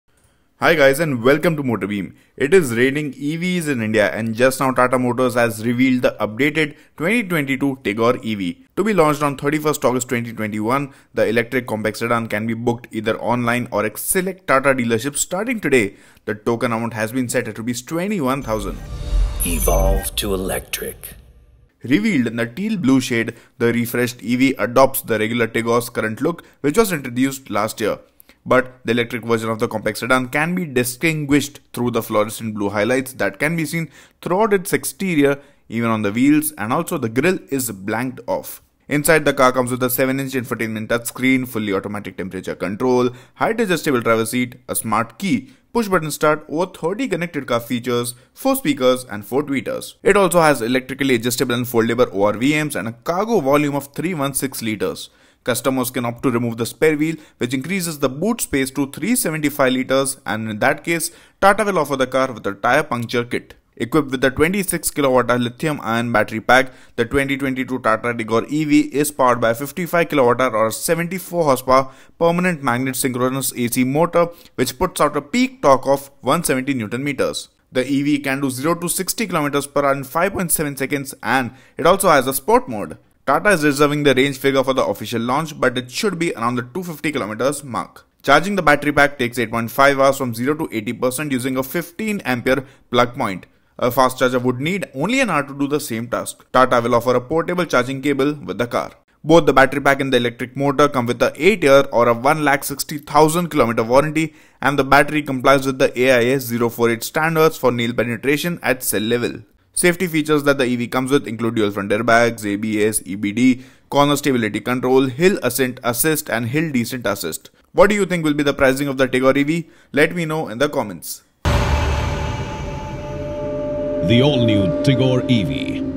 2022 Tata Tigor EV Revealed - Here's All You Need To Know | MotorBeam
The Tata Tigor EV has been officially revealed today ahead of its launch. The vehicle gets a major update mechanically while cosmetically it is based on the Tigor facelift that came out last year. Tata Motors will launch the Tigor EV on 31st August 2021.

Become a MotorBeamer and join the MBSquad